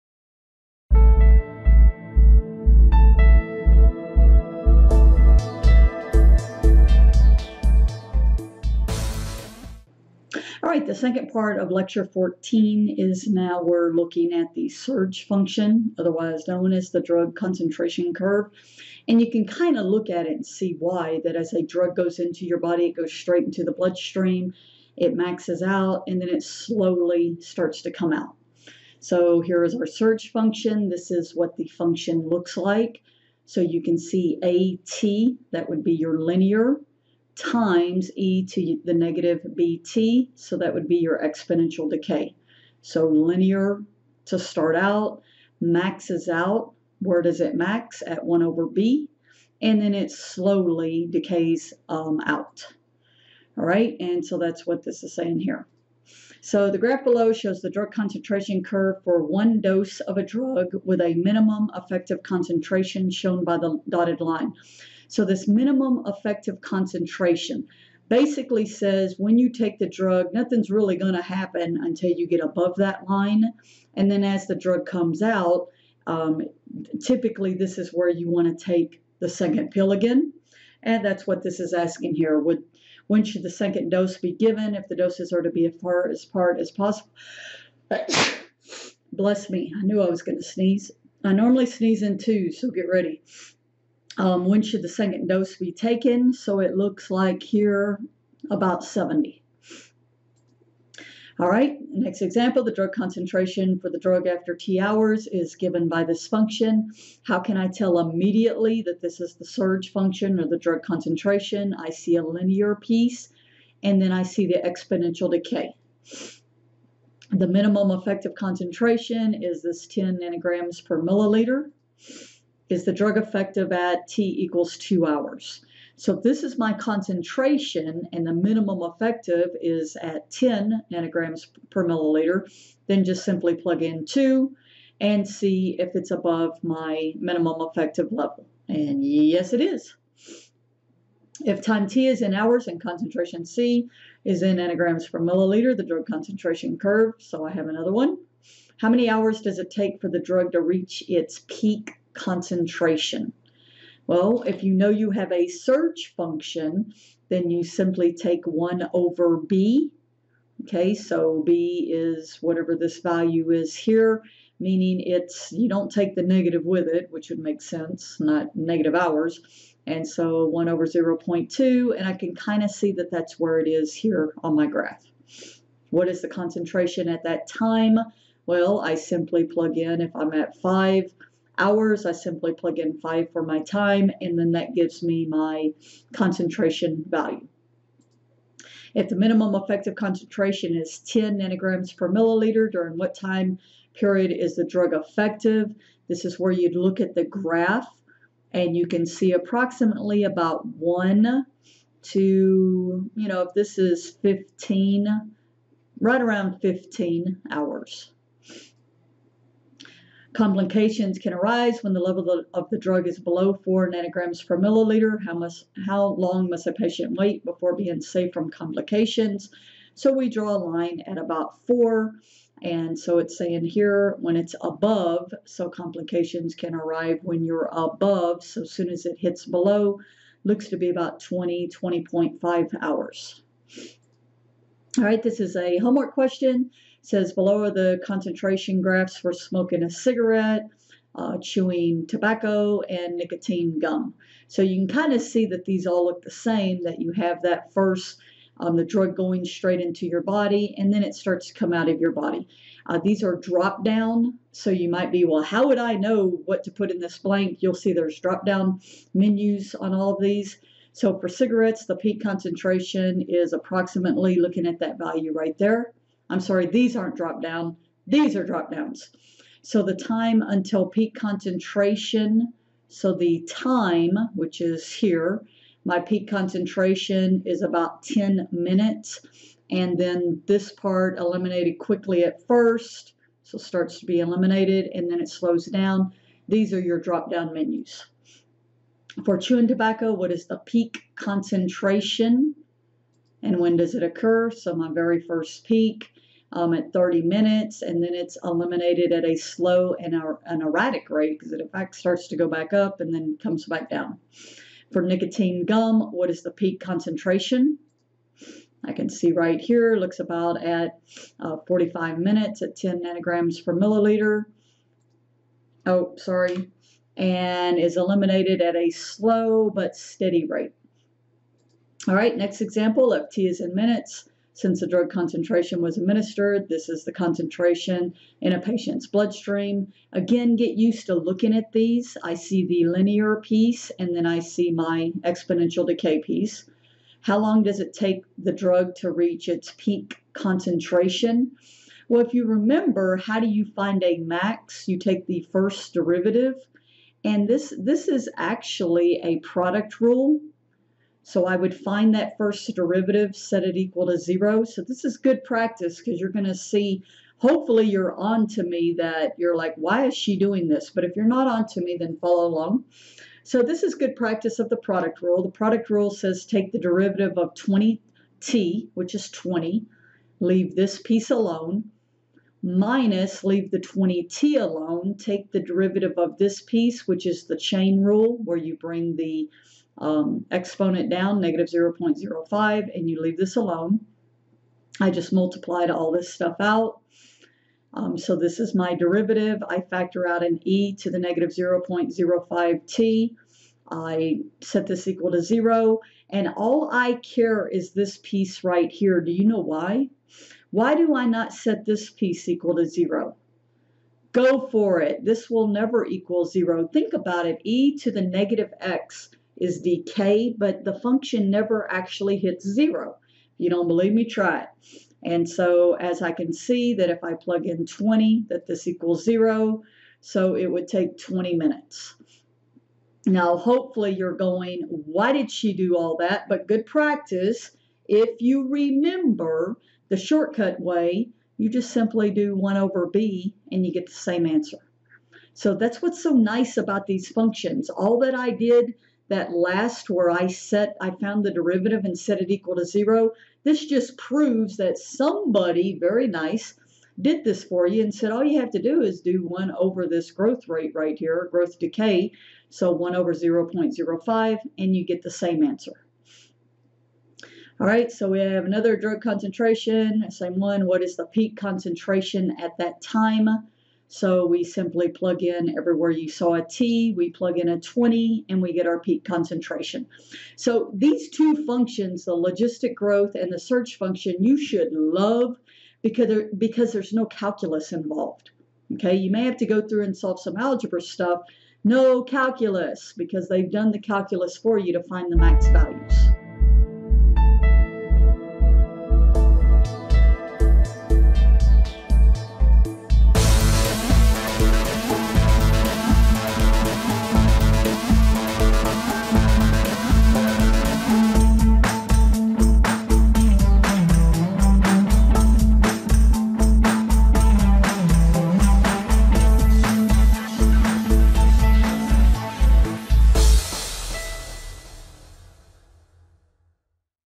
MAT1193 Lecture 14 Surge Function
Surge Function aka Drug Concentration Function